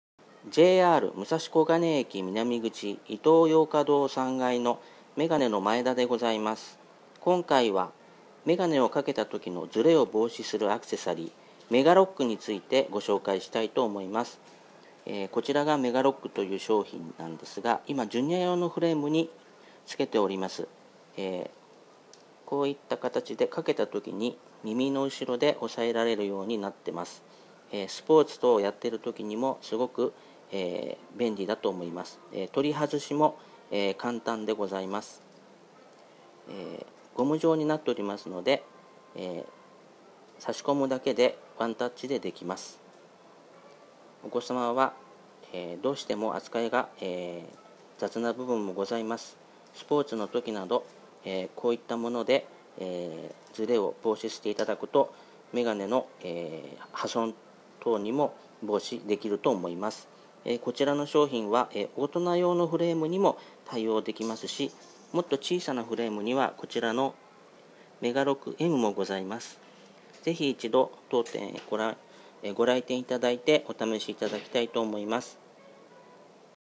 メガネのズレを防止するアクセサリー メガロックのご紹介 （武蔵小金井 メガネ）
JR中央線「武蔵小金井」駅南口イトーヨーカドー３階のメガネのマエダです。今回は、メガネのズレを防止するアクセサリーをご紹介します。装着が簡単で、大人用フレーム、ジュニアフレーム両方に対応可能です。